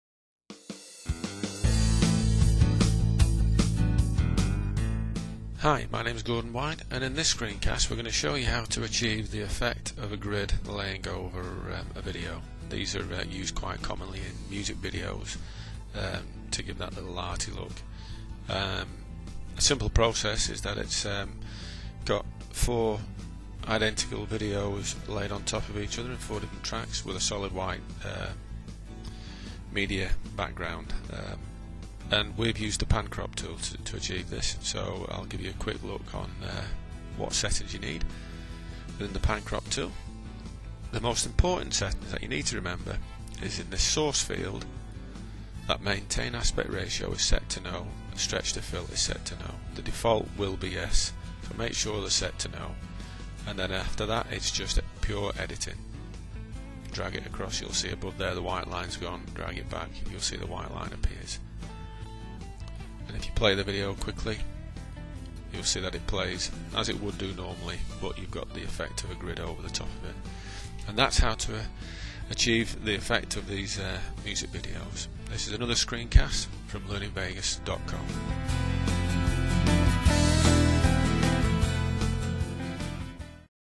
Create A Grid Look in Sony Vegas
or check out Editing Made Easy with Sony Vegas. This is how to create the look that a lot of music videos use. It appears that there are multiple screens or a grid is placed over it.